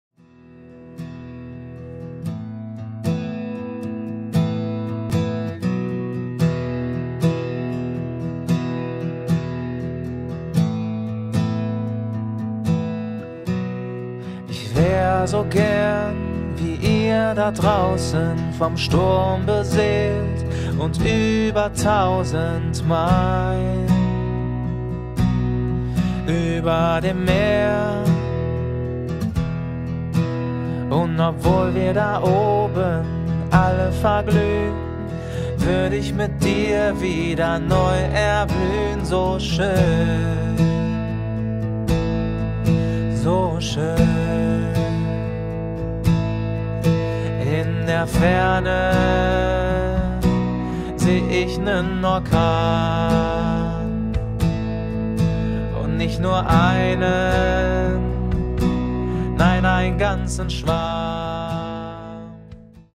CoachHaus Rheinbach Sebastian Landwehr und Wilko 20.01.2017
Ein kleiner Einblick vom Auftritt von Sebastian Landwehr und Wilko im CoachHaus in Rheinbach.